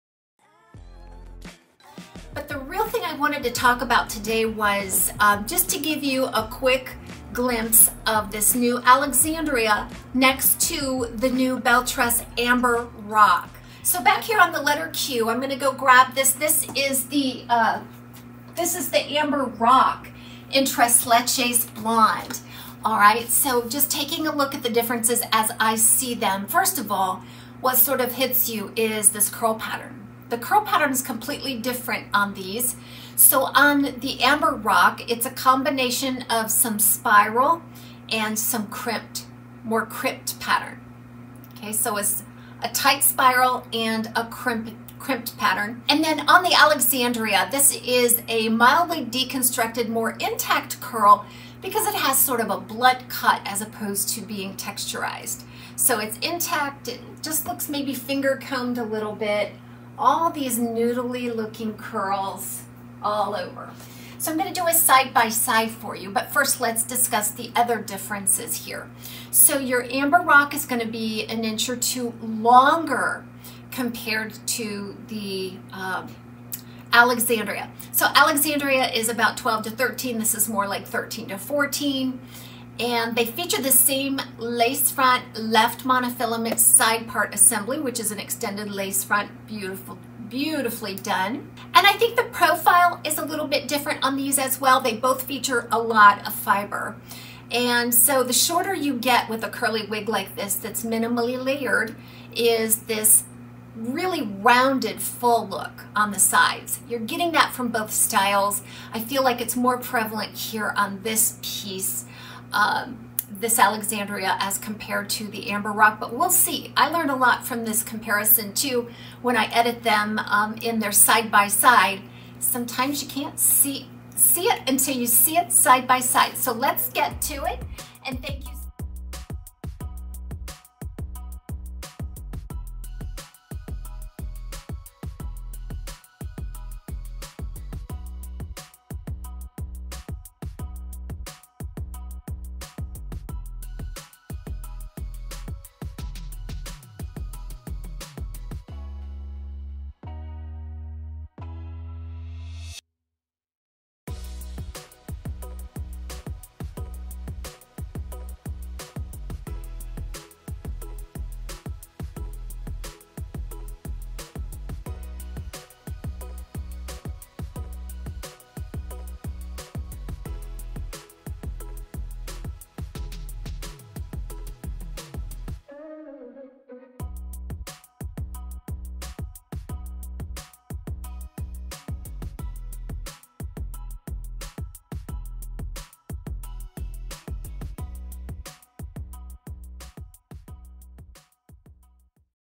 🔶Let's compare ALEXANDRIA VS AMBER ROCK🔶 Belle Tress WIGS🔶SEE them SIDE-BY-SIDE!
LET'S COMPARE Belle Tress Alexandria in English Toffee & Belle Tress Amber Rock.  Wigs are from my own collection. See below for links to the full length reviews of each of these styles! XOXO TAZ

🔹My WIG MEASUREMENT -  21.25” Circumference 6.5” from hairline to chin 3-4” from chin to collarbone 5’2.5” is my height and I have a medium frame

Produced and Filmed in 2023 by TAZS WIG CLOSET& TAZS WIG CHARM